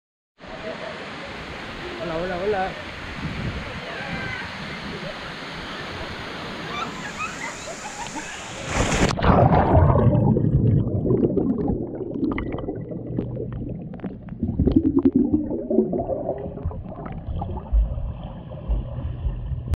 Agradece Por Todo
Cuando eres agradecido por lo que Dios te da así como el darte la vida y estar al lado de tu gente Papas y demás familia eso es grandioso.

Gracias